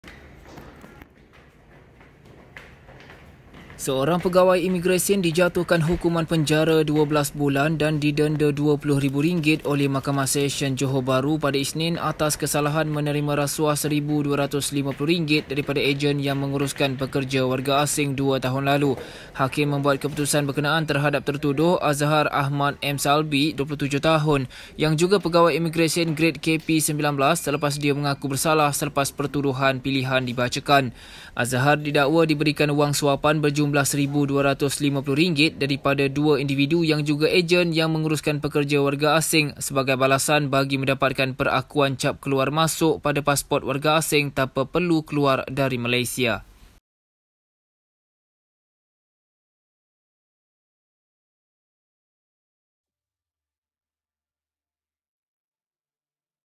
Rasuah: Pegawai Imigresen Gred KP19 mengaku salah
JOHOR BAHRU: Seorang pegawai Imigresen dijatuhkan hukuman penjara 12 bulan dan didenda RM20,000 oleh Mahkamah Sesyen, di sini, hari ini, atas kesalahan menerima rasuah RM1,250 daripada ejen yang menguruskan pekerja warga asing, dua tahun lalu.